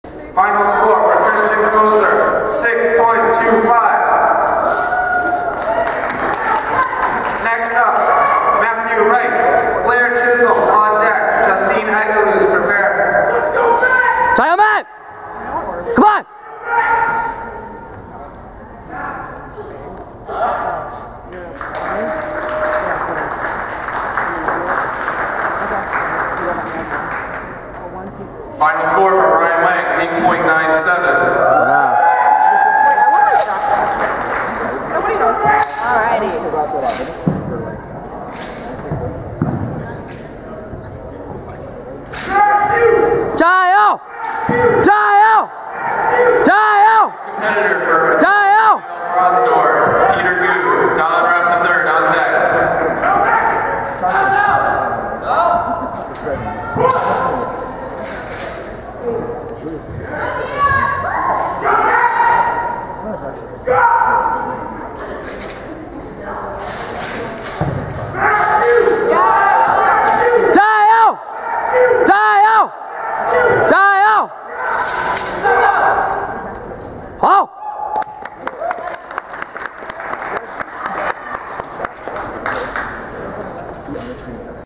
10th University Wushu Games Adult BS M Beg - Matthew Wright (Gold)
Matthew "Matt" Wright of UMBC competing in Adult Broadsword Male Beginner at the 10th University Wushu Games.  Placed 1st (Gold).

Taken at Ritchie Coliseum on November 22nd, 2014.